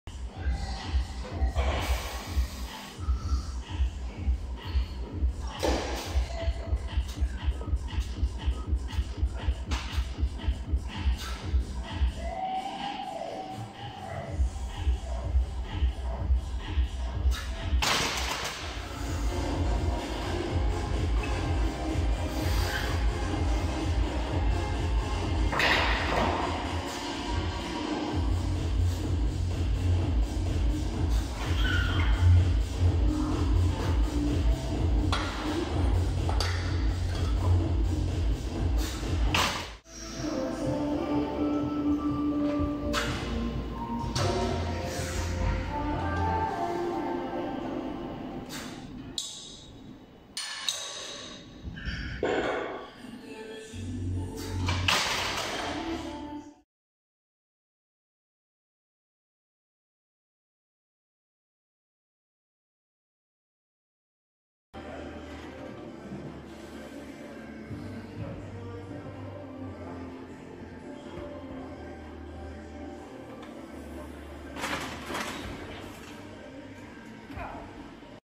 Tung Bänkpress på Stepin Piteå
Stepin Backcity Piteå 2021. Bänkpress 130-165 kg av en herre på 57 bast. Mitt lifetime max är en trea på 170 kg. Gjordes när jag var 48 år, på Öjeby AK 2012. Mitt bästa tävlingsresultat (och enda!) är 167,5 kg i klassisk bänk, på bänkpress-DM i Jokkmokk 2012 (finns på youtube). Jag blev totalt näst båste bänkpressare (på poäng) på det DM:et.